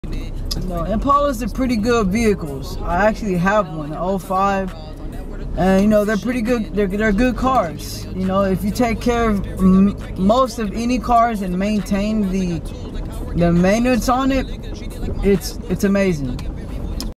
Impalas: The Perfect Blend of Style and Performance
Discover why Impalas are the ultimate vehicles for car enthusiasts. Learn about their top-notch performance and elegant design, backed by personal experience.